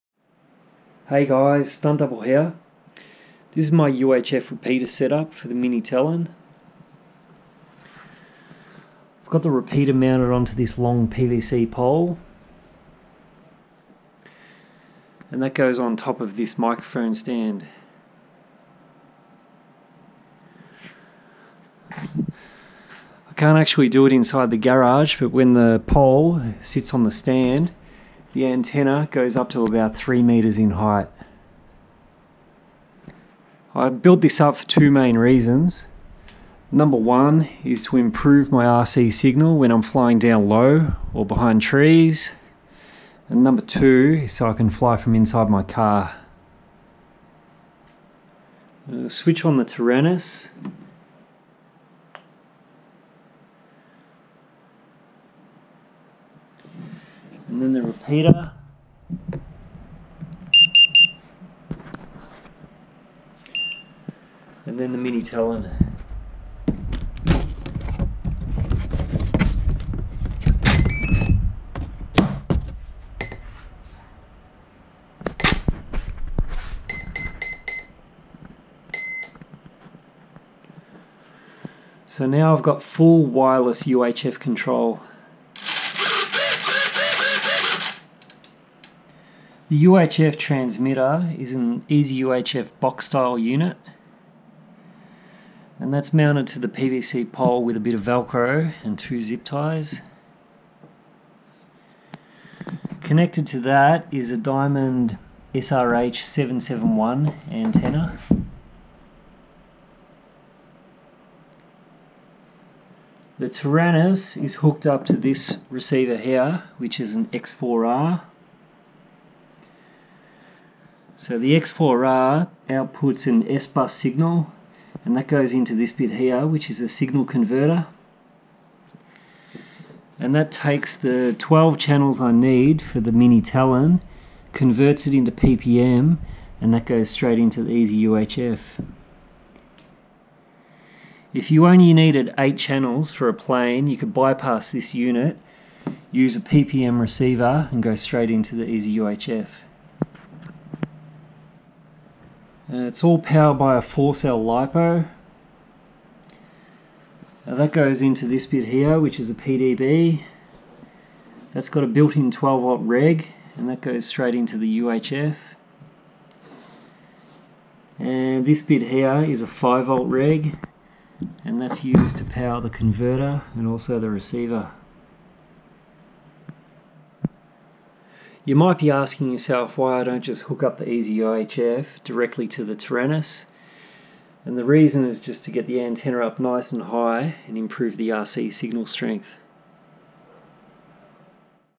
» UHF Repeater for the Mini Talon
This is my UHF repeater setup for the Mini Talon. The idea is to get the antenna up as high as possible to improve the RC signal and prevent failsafes when flying down low at distance or when flying from inside my car on a cold day.

The Taranis radio is connected to the EzUHF transmitter wirelessly using a Frsky X4R, Rmilec signal converter, PDB with 12v reg, D-sun step down bec & a 4S lipo.